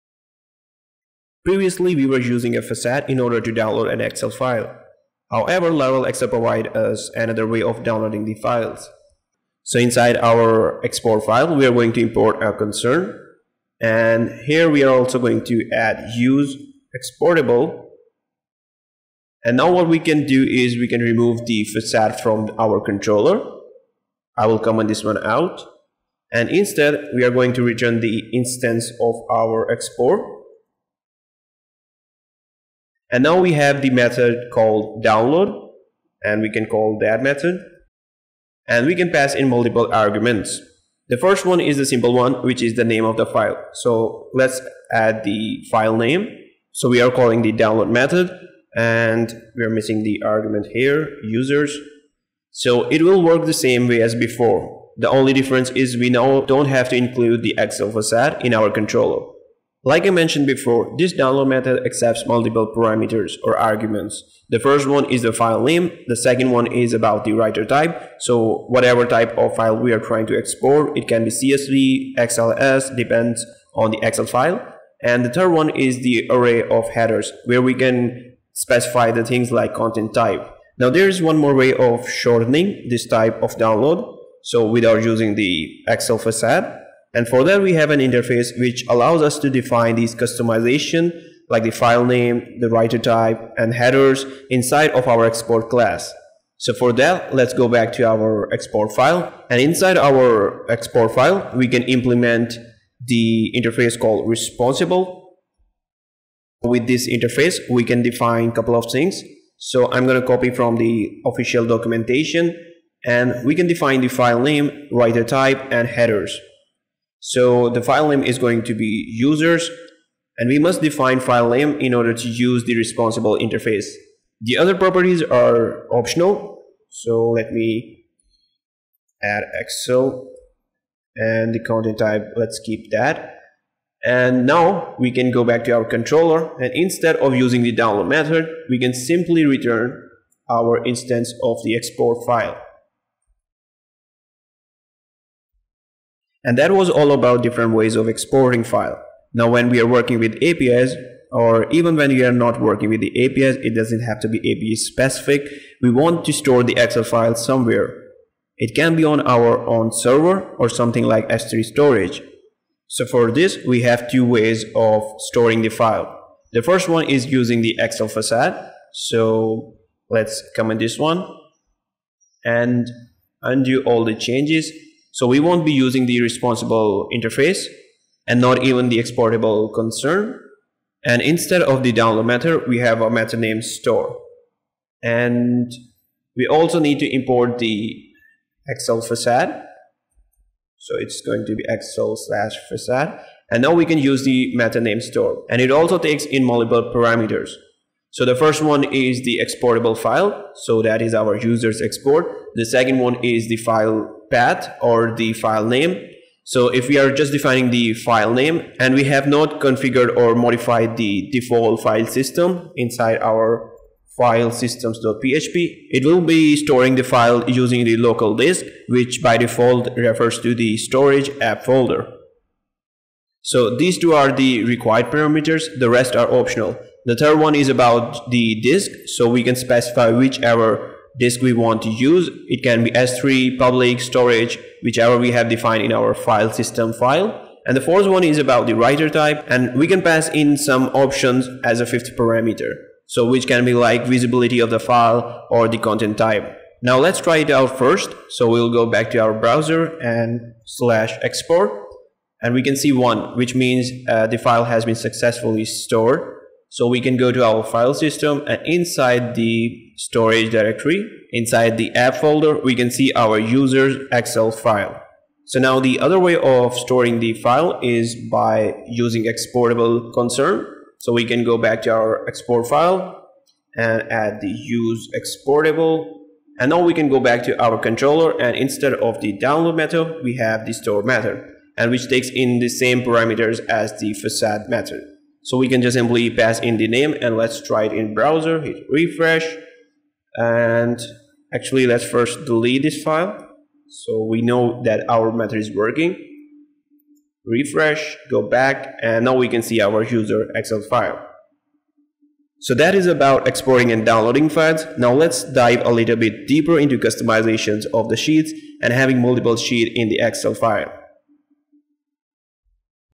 #07 - Laravel Excel - Exporting and Storing Excel Files
Laravel Excel Tutorial Series.

In this video, we'll be see how we can export Excel file and store it inside our storage.